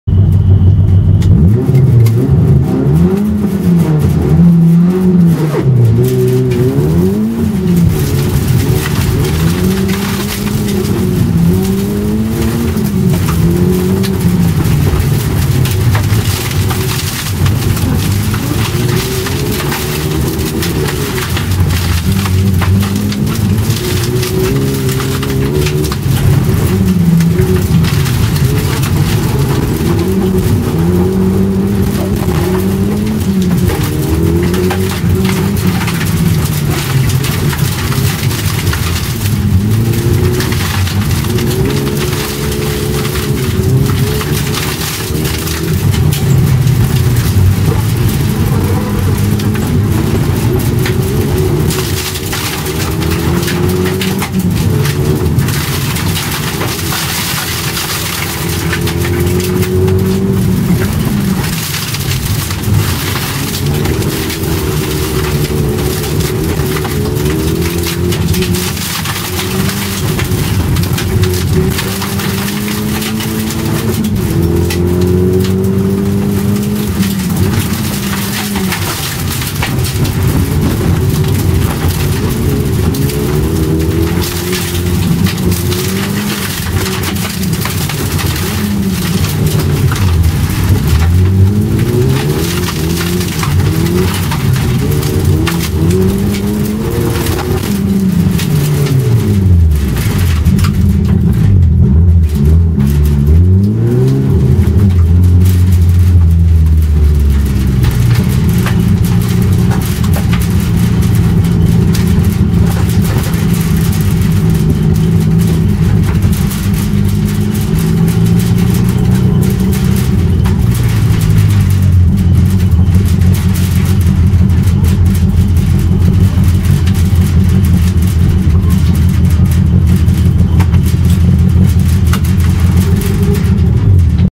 2012.5.13_近畿ダートトライアル＃３_いちろ車載１.MP4
スタートでギア入らず。。。前半グダグダ。